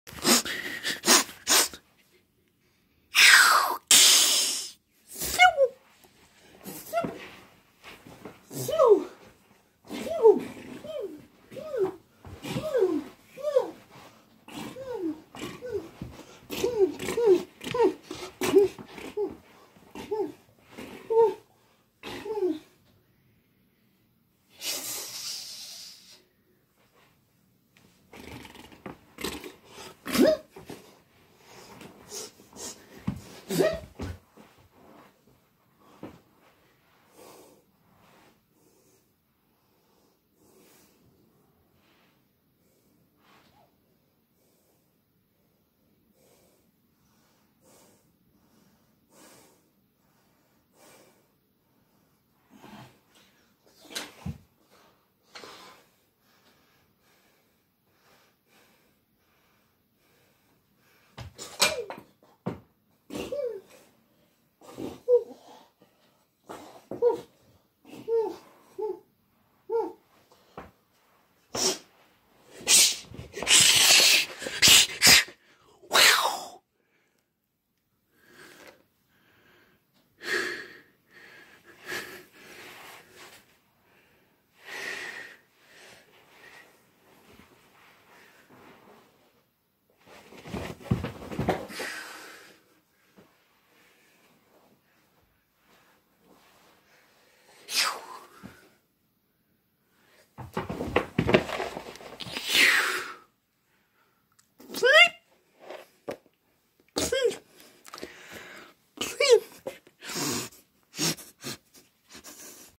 I ACTED LIKE A MONGOOSE FOR 1 MINUTE AND 52 SECONDS!!!! ACT TINGS EPISODE 8
Hey guys, so in all my Act Tings, I have not shown my skill to act as an animal yet. Since going to Drama School, I have been studying what it is like to act a Mongoose. This is what I have done. Enjoy😁.